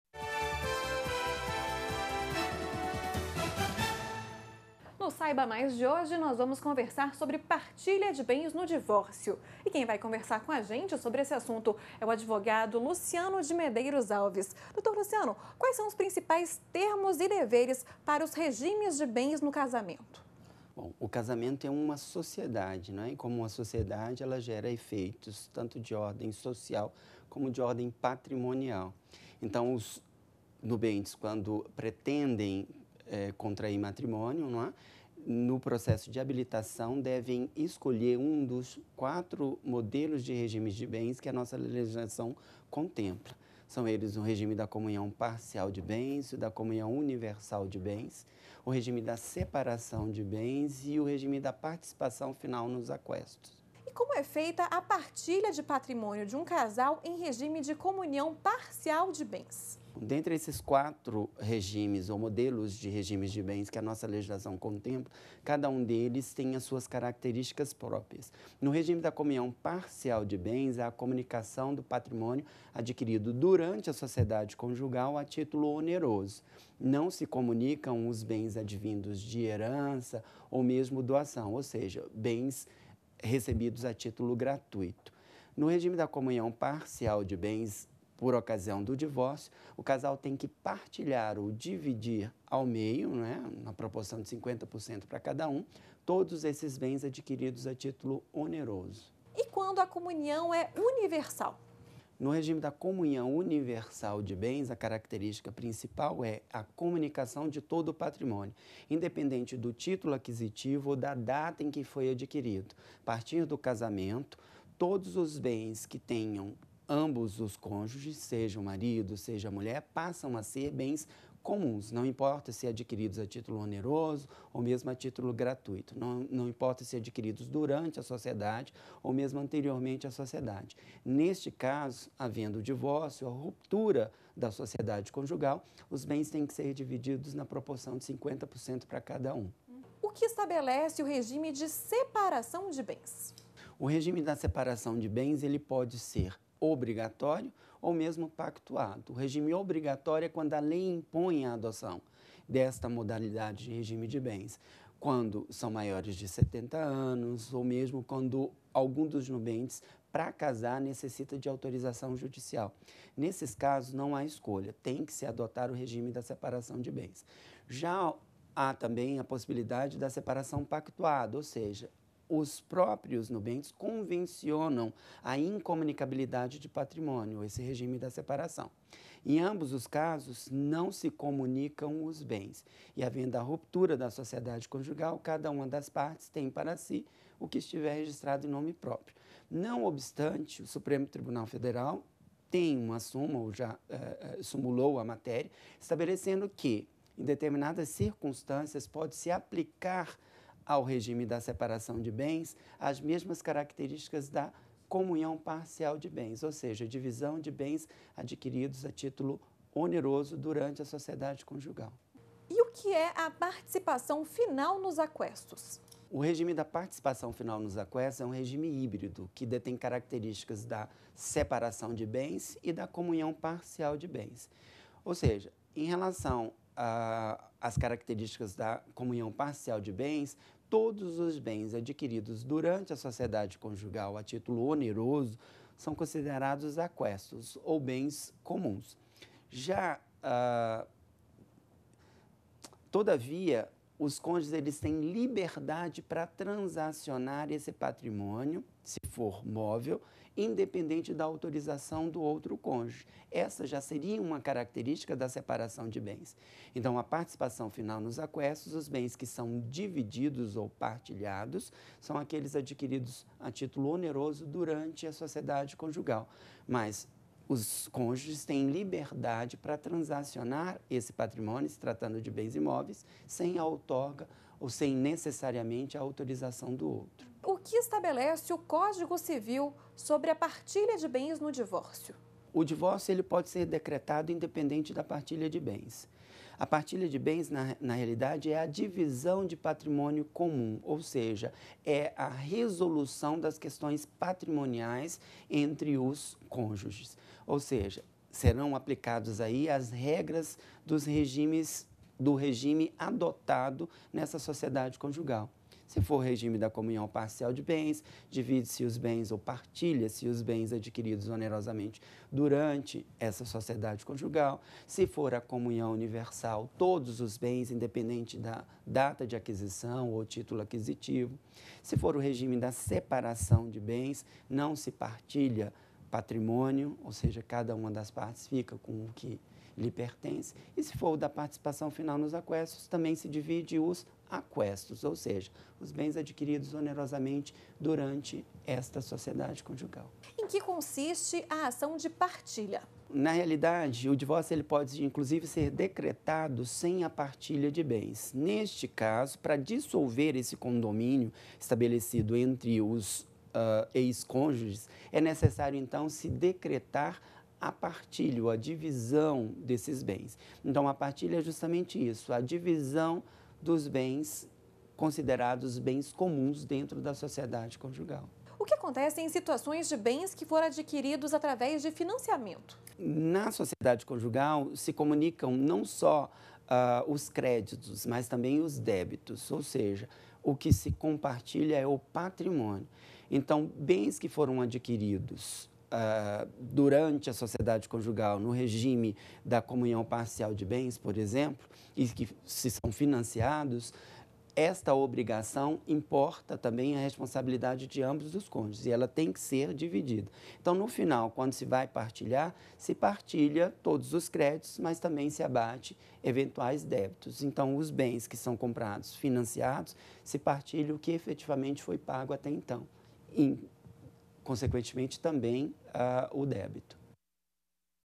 Saiba Mais - Partilha de bens
Confira no quadro "Saiba Mais" uma entrevista com o especialista em direito processual civil Luciano de Medeiros Alves  sobre a partilha de bens no divórcio.